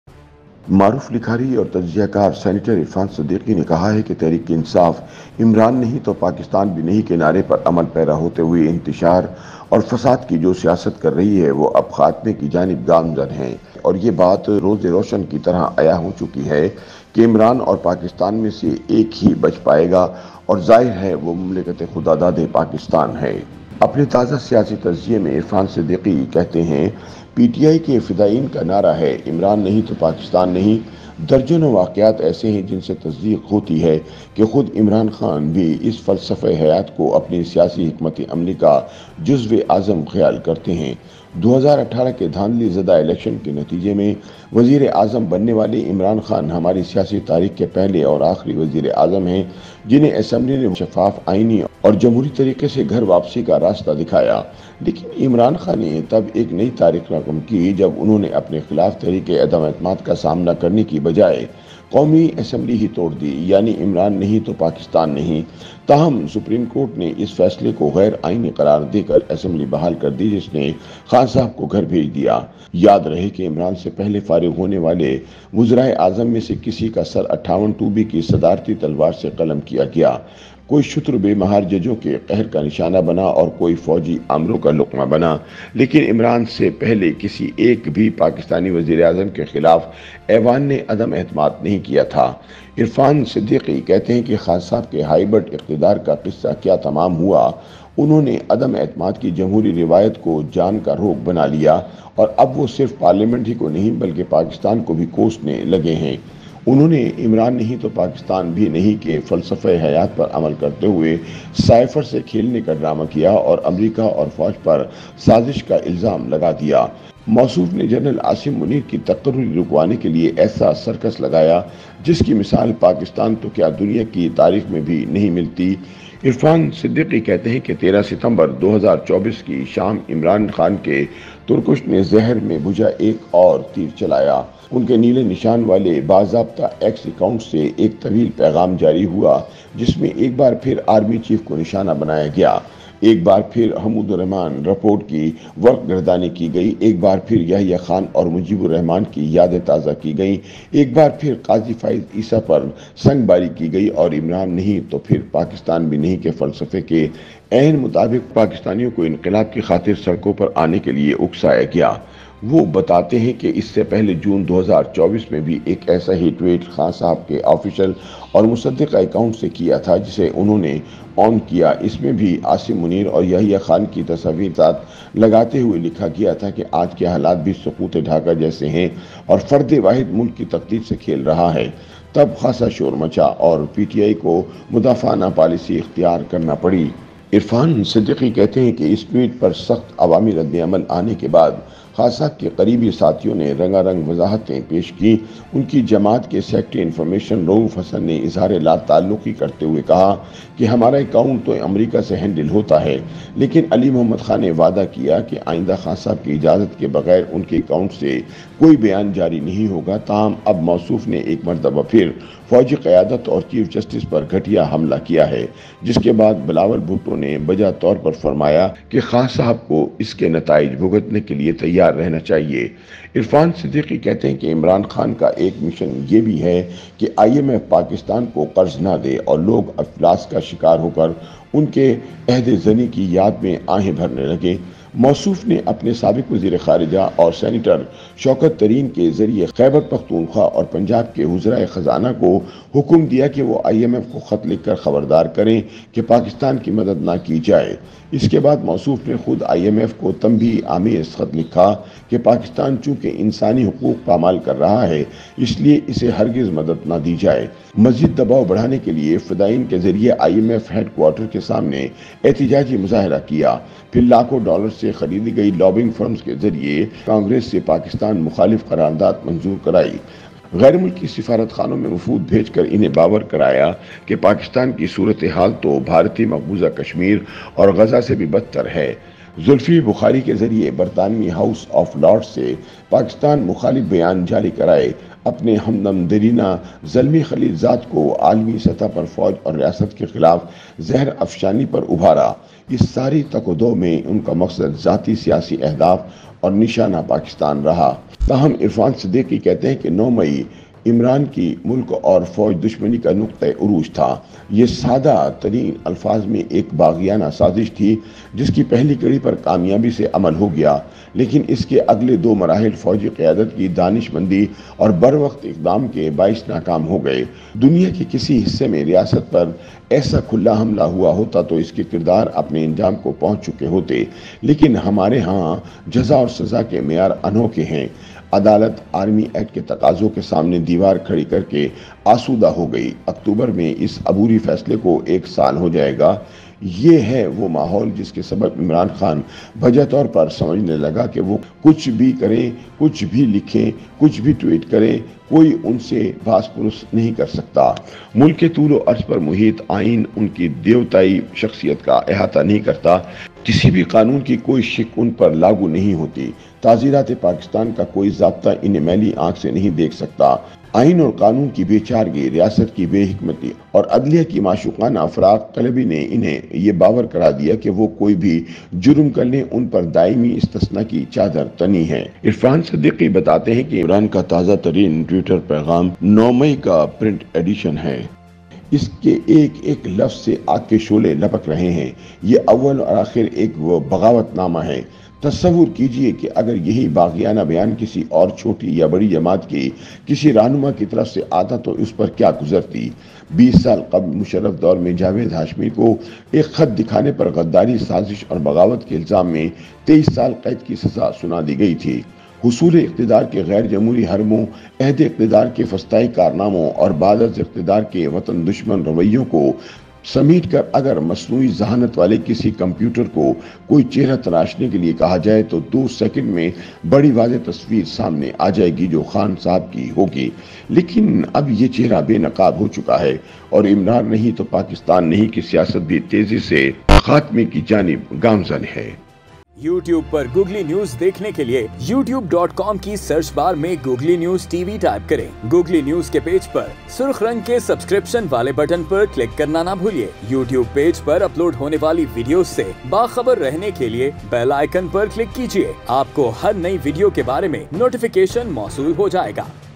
"Imran Nahin Tau Pakistan Nahin" Kei Siasat Khatam Kiyun Honay Wali Hai? : Irfan Siddiqui | Googly
"Imran Nahin Tau Pakistan Nahin" Kei Siasat Khatam Kiyun Honay Wali Hai? : Irfan Siddiqui